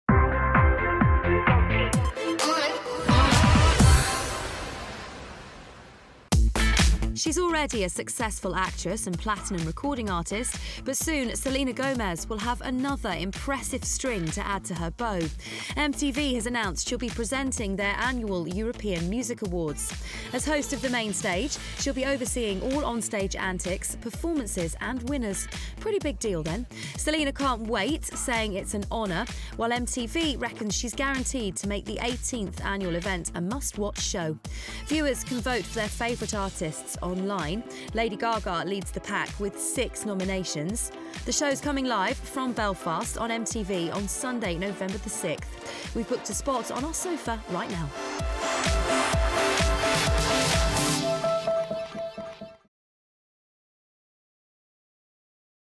Selena Gomez to host MTV EMAs
Actress Selena Gomez will take over hosting duties for the 2011 MTV Europe Music Awards in Belfast. Test